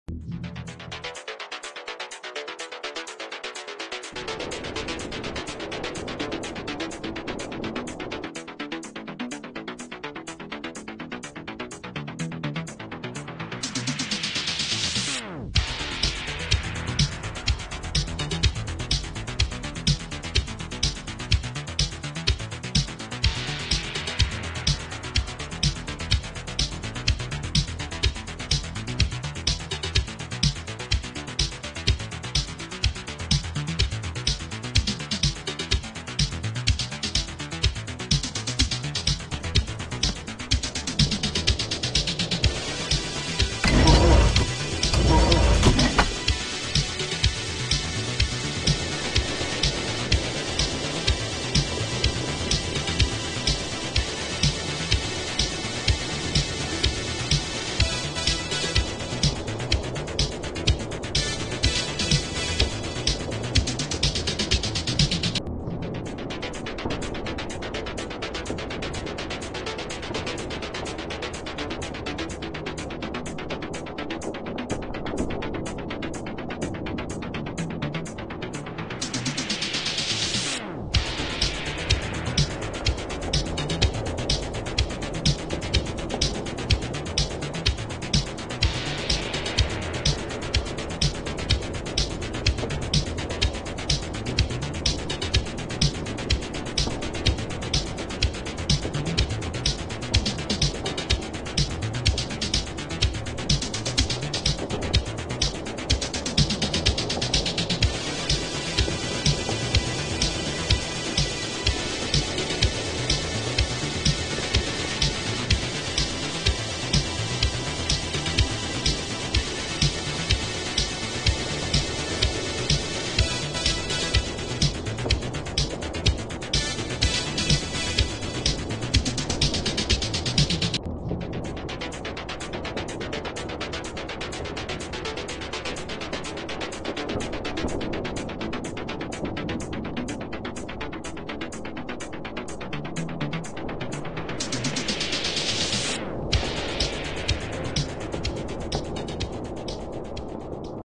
Keplerians news and Evil nun 2 new update secret ? 🤔
Keplerians news and Evil nun 2 new update theory
Kn latest Keplerians news they revealed some book photos of Evil nun 2 new update and I try and find what happened to the chickens and what will be the evil nun 2 secret ending